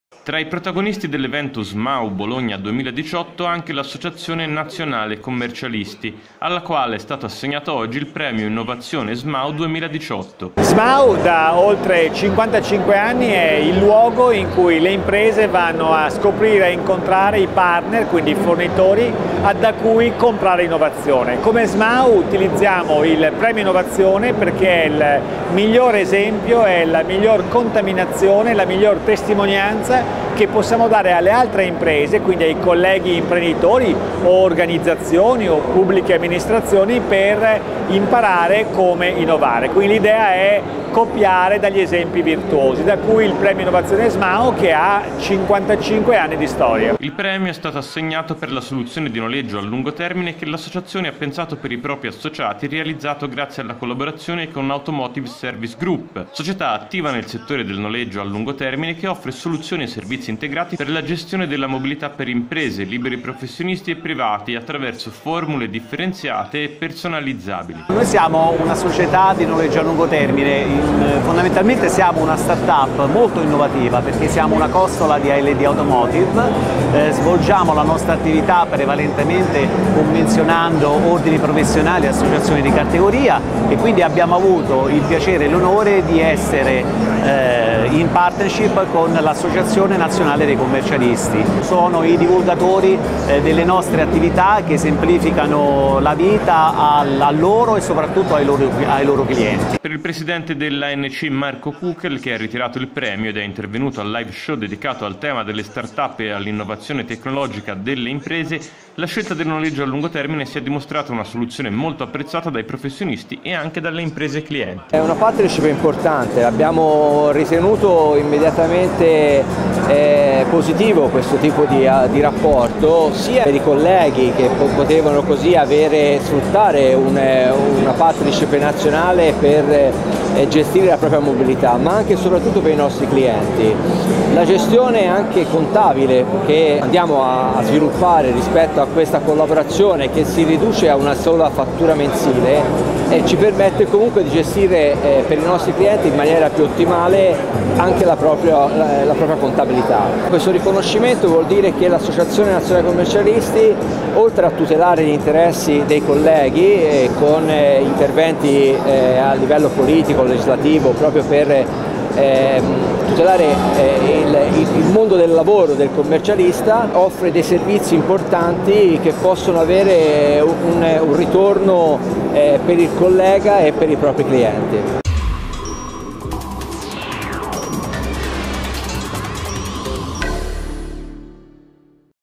L' Associazione Nazionale Commercialisti riceve il premio SMAU Innovazione 2018
Tra i protagonisti dell’evento SMAU Bologna di quest’anno c’è anche l’Associazione Nazionale Commercialisti alla quale è stato assegnato oggi il premio Innovazione SMAU 2018 per la soluzione di noleggio a lungo termine che l’Associazione ha pensato per i propri associati e realizzato grazie alla collaborazione con Automotive Service Group, società attiva nel settore del noleggio a lungo termine che offre soluzioni e servizi integrati per la gestione della mobilità per imprese, liberi professionisti e privati, attraverso formule differenziate e personalizzabili.

Per il Presidente dell’ANC Marco Cuchel - che ha ritirato il premio ed è  intervenuto al live show dedicato al tema delle startup e all'innovazione tecnologica delle imprese, la scelta del noleggio a lungo termine si è dimostrata una soluzione molto apprezzata dai professionisti e anche dalle imprese clienti.